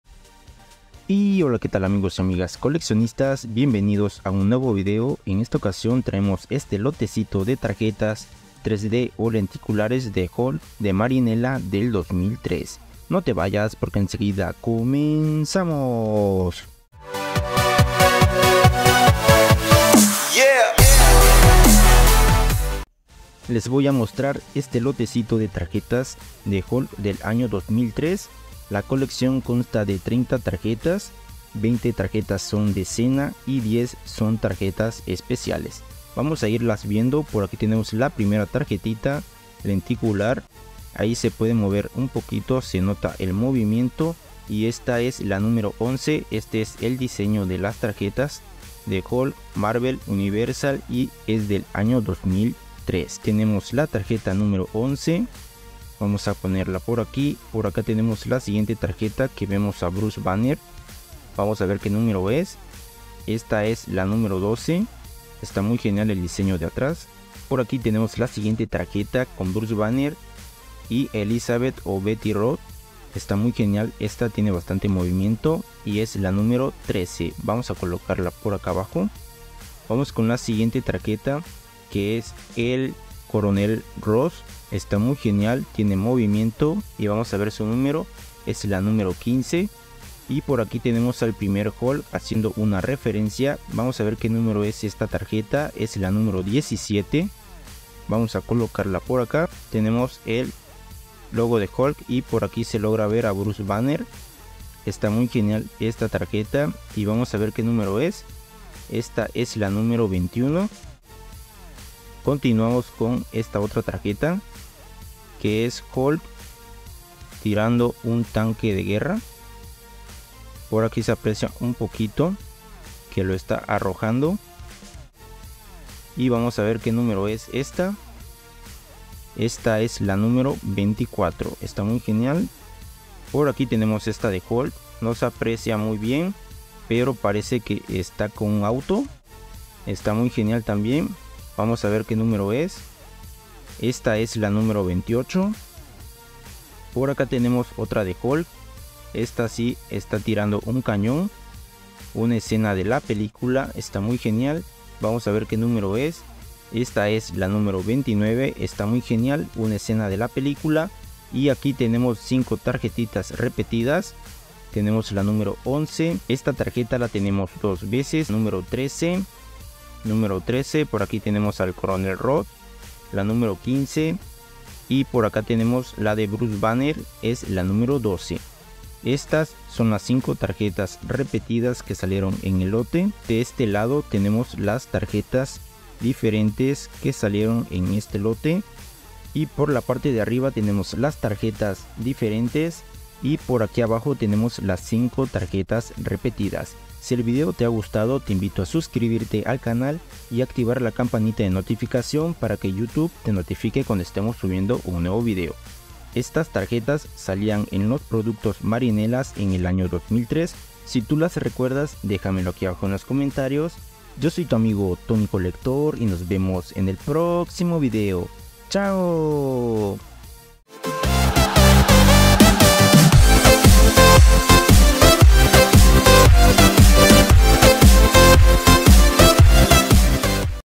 Tarjetas de Hulk de Marinela 2003
Acompáñame a ver este pequeño lote de tarjetas lenticulares de la Película de Hulk del año 2003, las podías encontrar en los productos Marinela